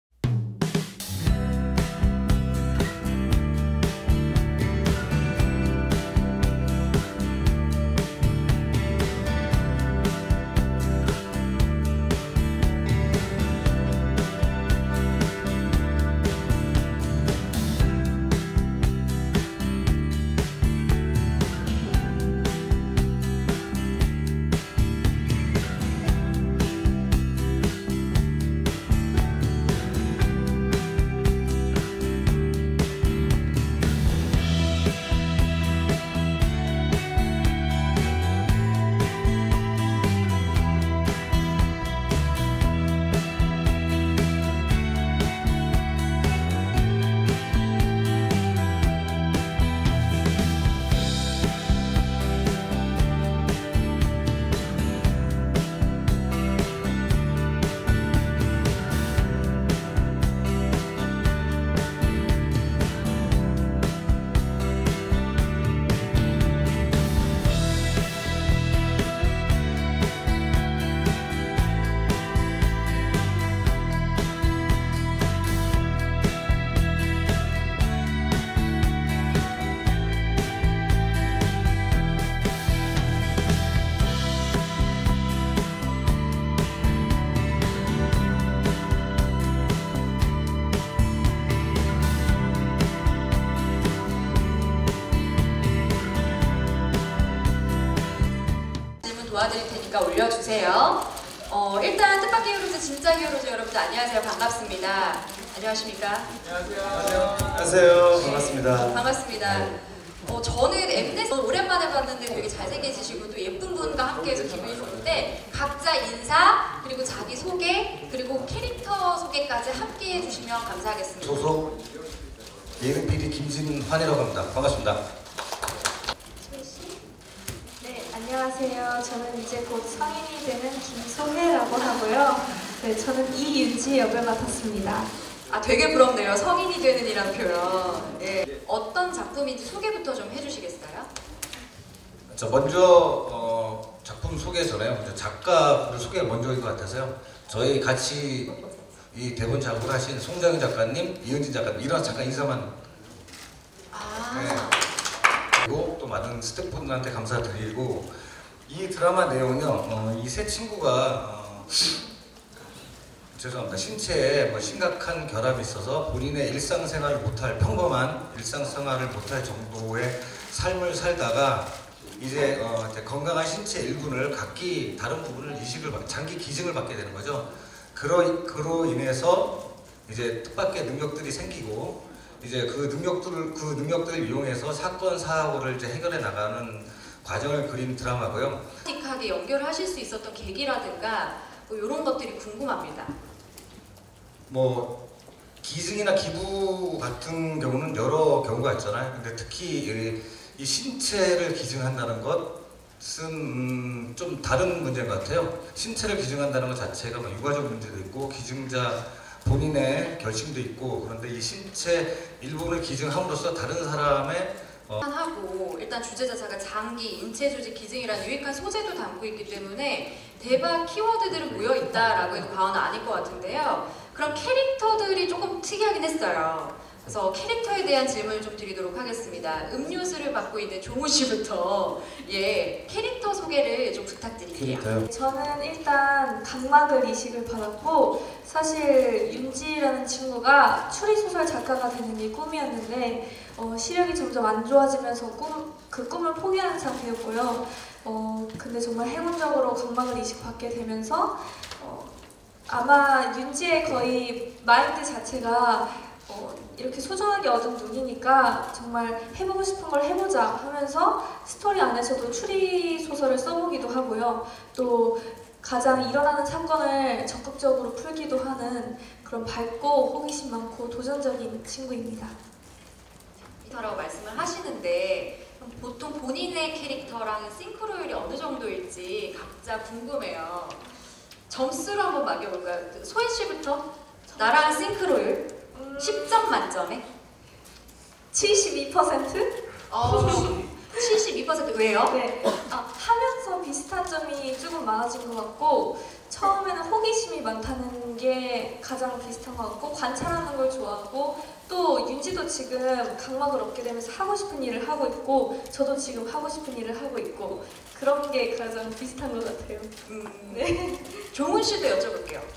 김소혜 세로 직캠 '이렇게 사랑스런 펭귄 보셨나요?'(아이오아이,kim sohye,웹드라마 '뜻밖의 히어로즈' 제작발표회)
13일 오후 서울 상암동 스탠포드 호텔에서 열린 네이버TV 웹드라마 '뜻밖의 히어로즈' 제작발표회에 이민혁, 김소혜, 최종훈, 박하나가 참석했다.